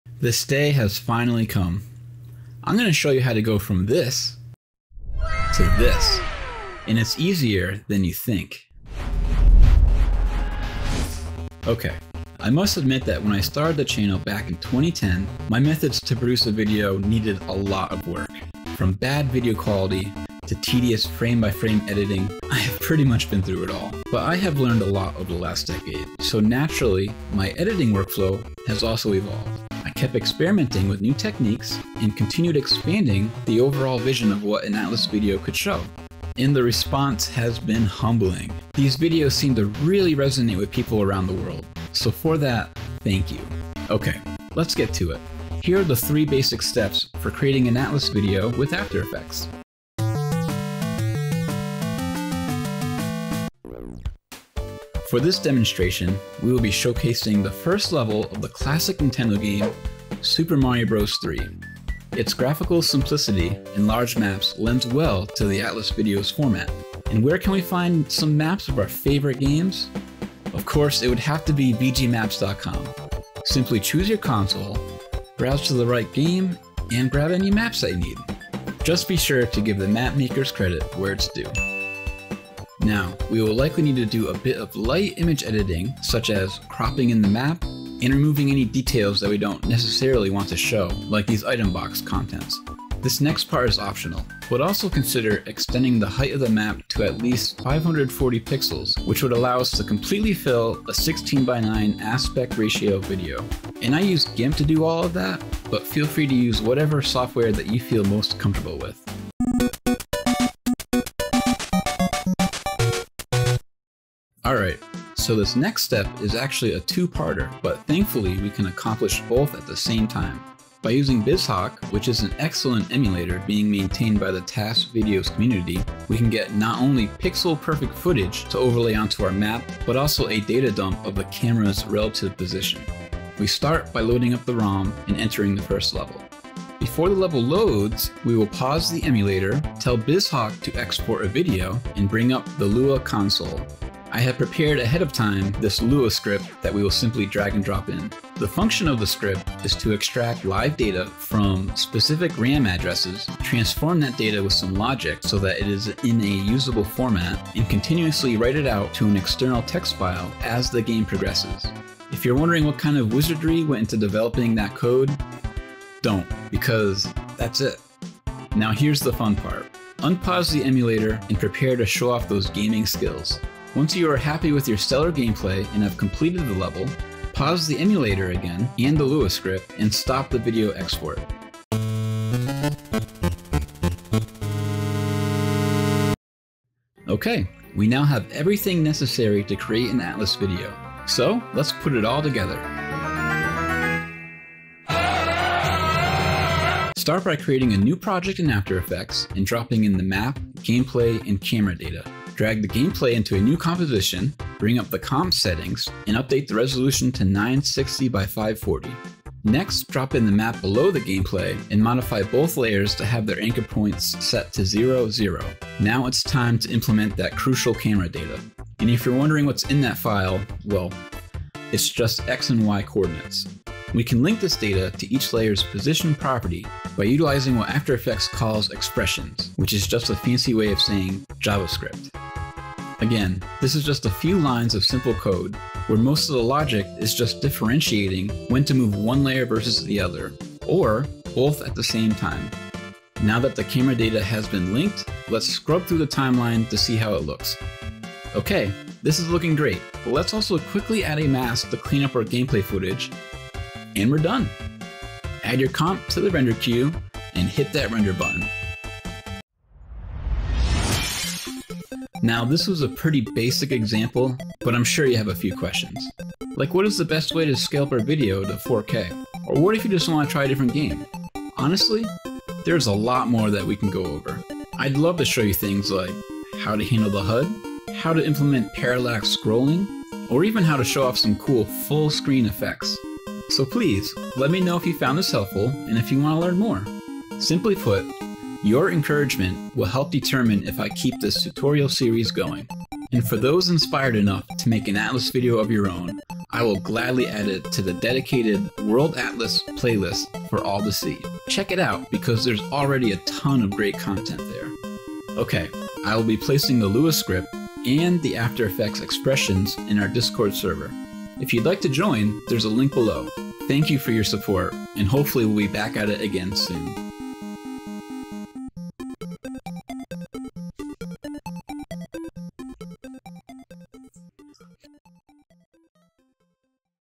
How to create Atlas Videos with After Effects 🧙
Here are my 3 basic steps to create an Atlas Video with After Effects 🥳